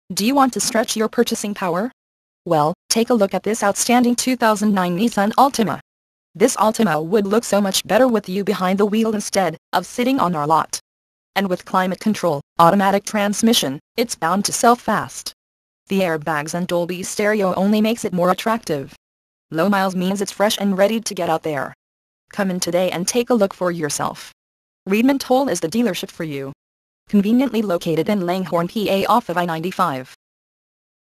Used 2009 Nissan Altima Langhorne PA
Year: 2009 Make: Nissan Model: Altima Trim: 2.5 SL Engine: 4 Cyl. Transmission: Automatic Color: Black Mileage: 25326 Address: Reedman-Toll Autoworld 1700 East Lincoln Highway Langhorne PA